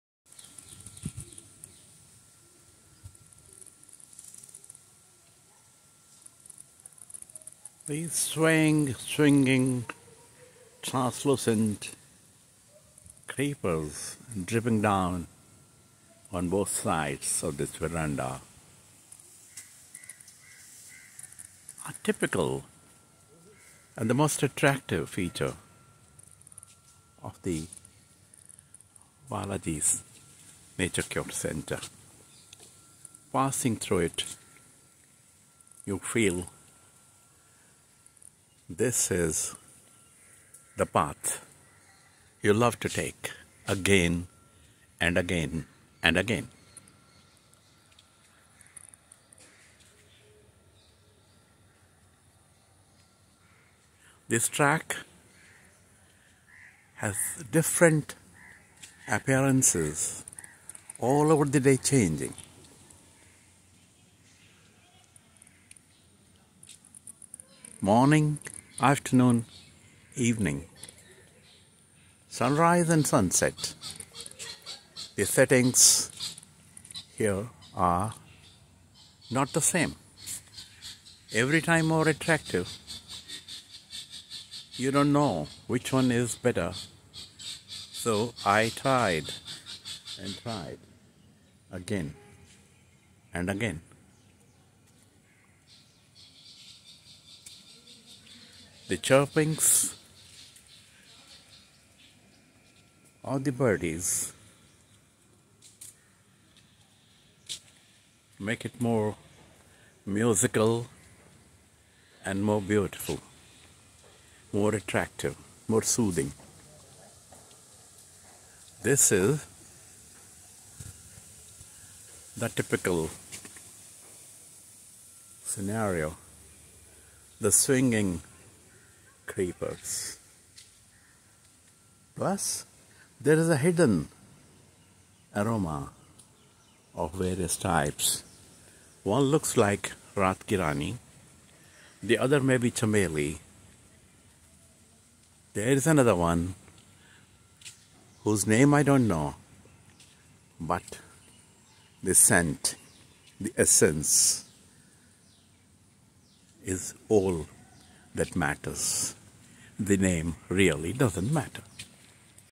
Swinging creepers of Balaji Naturopathy Centre
Track from the Sawak Sadan ( patient guest house) to various natural treatment centres goes through a verandah with these swinging creepers with sunlight filtering through. Scene changes hour to hour, the aroma of the flowers with fragrance just passes by. All visitors enjoy this partition between two halves the Balaji centre.